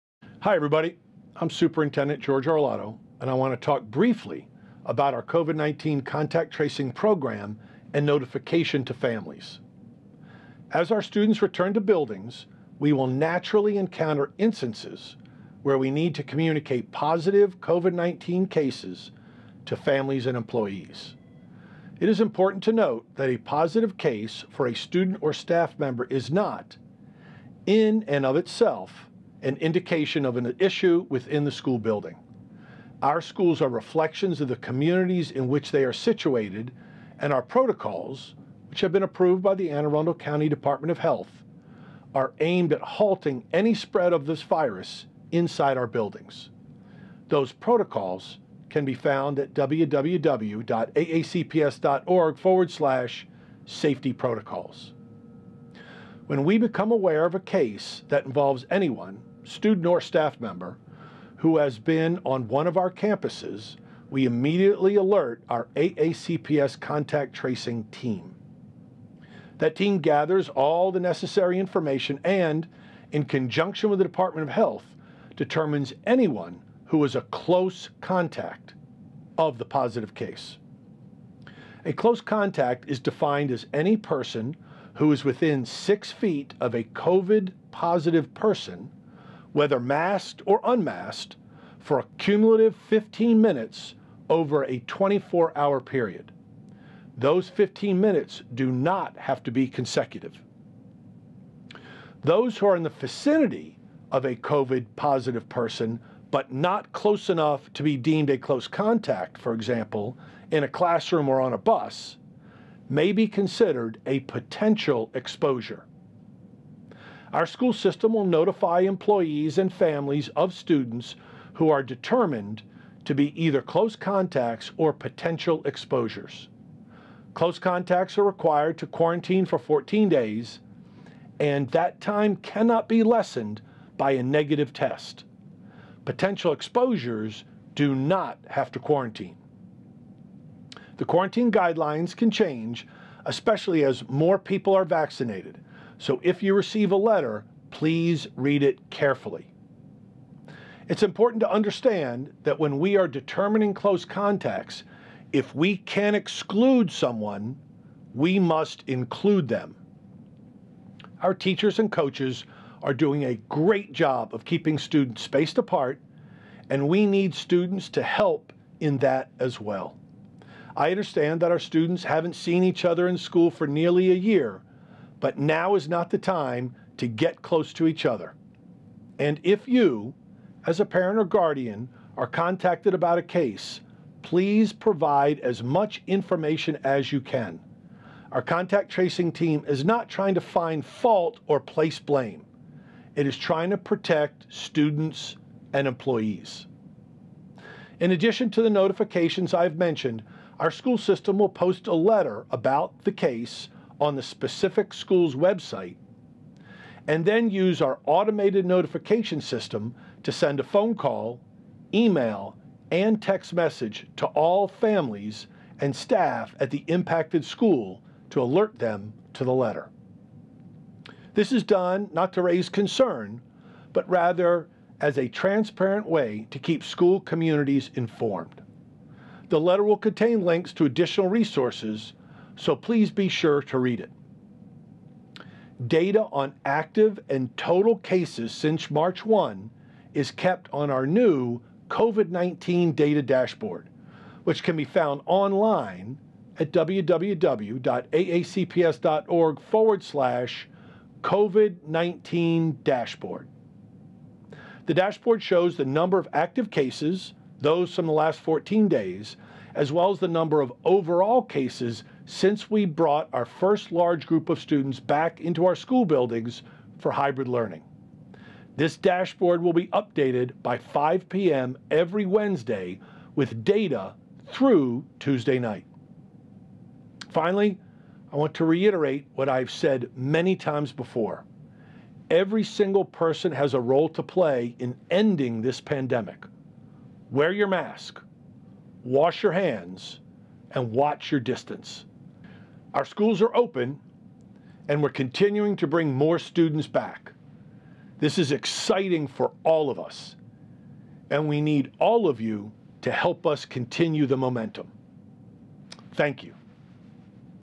Contact Tracing and COVID-19 Communication in AACPS
Hi everybody, I’m Superintendent George Arlotto and I want to talk briefly about our COVID-19 contact tracing program and notification to families.

As our students return to buildings, we will naturally encounter instances where we need to communicate positive COVID-19 cases to families and employees. It is important to note that a positive case for a student or staff member is not, in and of itself, and indication of an issue within the school building. Our schools are reflections of the communities in which they are situated, and our protocols, which have been approved by the Anne Arundel County Department of Health, are aimed at halting any spread of this virus inside our buildings.

When we become aware of a case that involves anyone – student or staff member – who has been on one of our campuses, we immediately alert our AACPS Contact Tracing Team. That team gathers all the necessary information and, in conjunction with the Department of Health, determines anyone who was a “close contact” of the positive case.

A close contact is defined as any person who is within 6 feet of a COVID-positive person, whether masked or unmasked, for a cumulative 15 minutes over a 24-hour period. Those 15 minutes do not have to be consecutive.

Those who are in the vicinity if a COVID-positive person but not close enough to be deemed a close contact – for example, in a classroom or on a bus – may be considered a “potential exposure.”

Our school system will notify employees and families of students who are determined to be either close contacts or potential exposures. Close contacts are required to quarantine for 14 days, and that time cannot be lessened by a negative test. Potential exposures do not have to quarantine.

The quarantine guidelines can change, especially as more people are vaccinated, so if you receive a letter please read it carefully.

It is important to understand that when we are determining close contacts, if we can’t exclude someone, we must include them. Our teachers and coaches are doing a great job of keeping students spaced apart, and we need students to help in that as well. I understand that our students haven’t seen each other in school for nearly a year, but now is not the time to get close to each other. And if you, as a parent or guardian, are contacted about a case, please provide as much information as you can. Our contact tracing team is not trying to find fault or place blame. It is just trying to protect students and employees.

In addition to the notifications I have mentioned, our school system will post a letter about the case on the specific school’s website and then use our automated notification system to send a phone call, email, and text message to all families and staff at the impacted school to alert them to the letter. This is done not to raise concern, but rather as a transparent way to keep school communities informed. The letter will contain links to additional resources, so please be sure to read it.

Data on active and total cases since March 1 is kept on our new COVID-19 Data Dashboard, which can be found The dashboard shows the number of active cases – those from the last 14 days – as well the number of overall cases since we brought our first large group of students back into our school buildings for hybrid learning. This dashboard is updated by 5 p.m. every Wednesday with data through Tuesday night.

Finally, I want to reiterate what I have said many times before: Every single person has a role to play in ending this pandemic. Wear your mask. Wash your hands. Watch your distance.

Our schools are open, and we’re continuing to bring more students back. This is exciting for all of us, and we need all of you to help us continue the momentum.